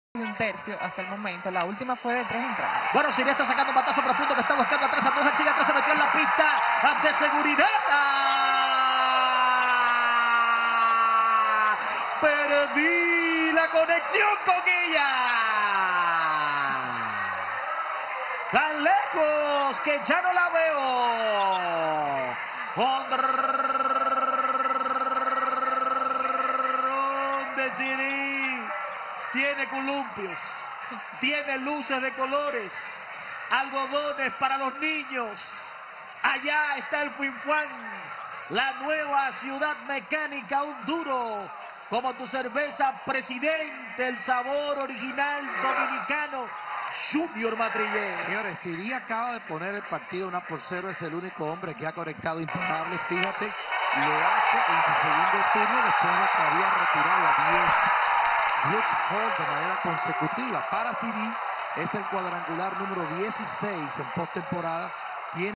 Increíble Jonrón de Jose Siri en el Round Robin! 🔥 | Liga Dominicana de Béisbol🚀💥🥎
Increíble Jonrón de Jose Siri en el Round Robin! 🔥 | Liga Dominicana de Béisbol🚀💥🥎. Video: Lidom Gigantes del Cibao.